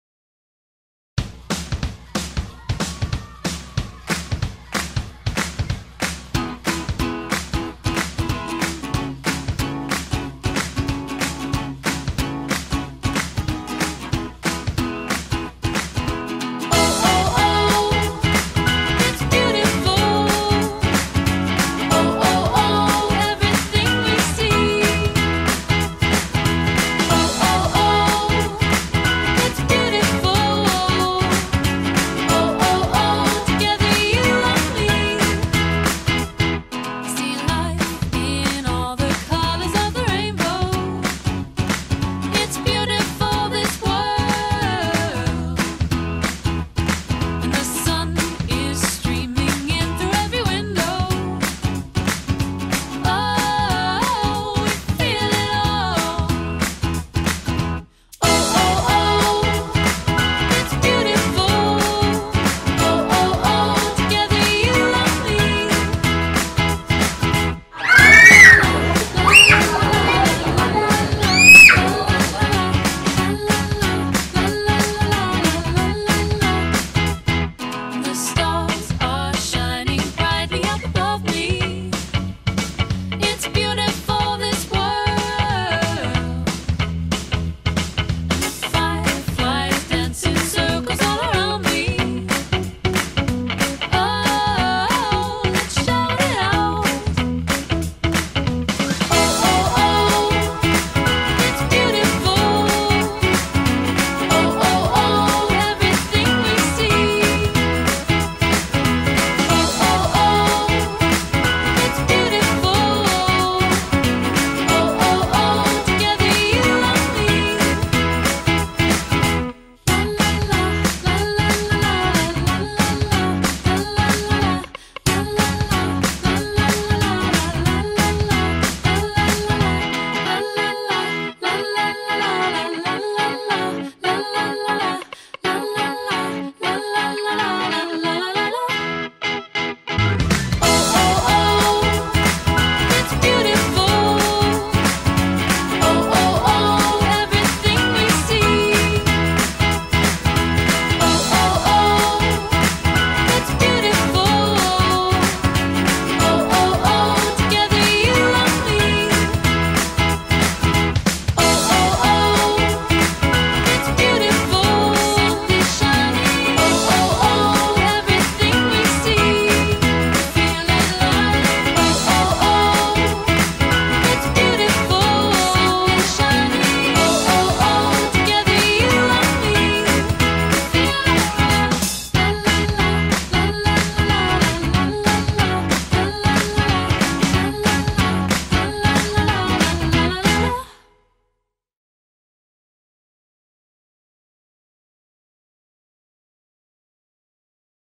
Trailer - Mariana 6 anos
Uma festa linda e muuuito divertida. Não há nada melhor do que comemorar cercado por pessoas especiais, repletos de alegria.

Filmagem: Singular Films | Fotografia: MLM Fotografia | Buffet: Conte Outra Vez